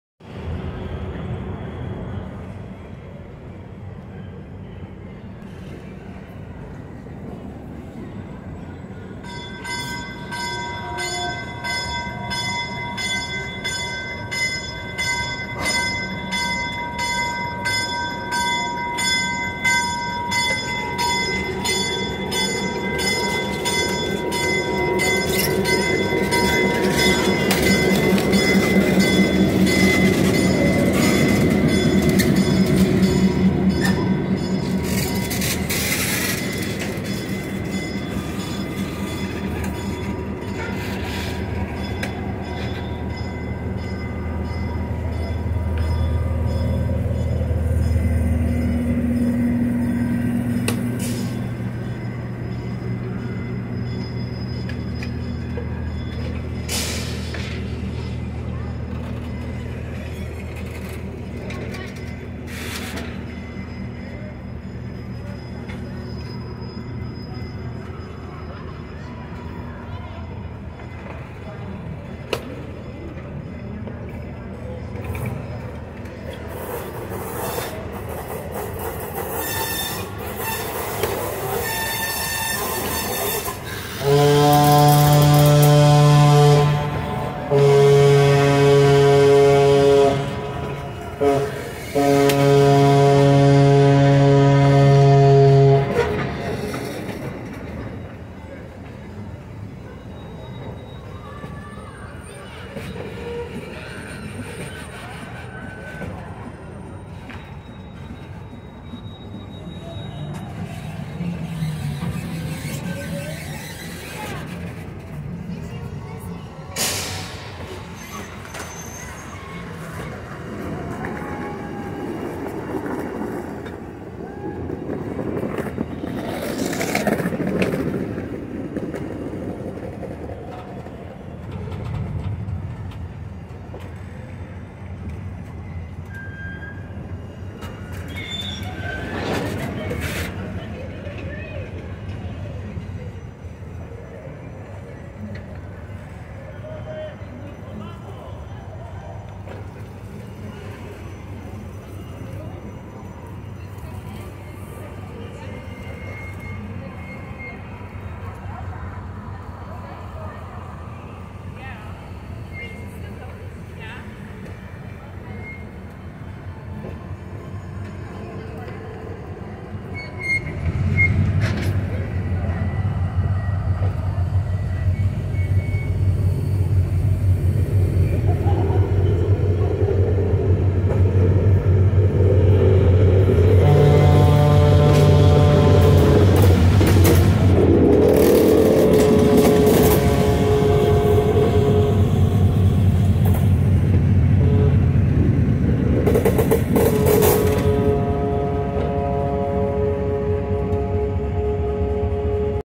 Western Pacific 913 Leads CSRM Polar Express Excursion Train @ Sacramento CA 11/28/2021
My last catch for the evening is the western Pacific 913 leading the final run of the California State Railroad Museum Polar Express Excursion Train on the ex SP Walnut Grove branch line at 8:09pm

Engine
WP 913 EMD (F7A)
SP 6051 EMD (E9)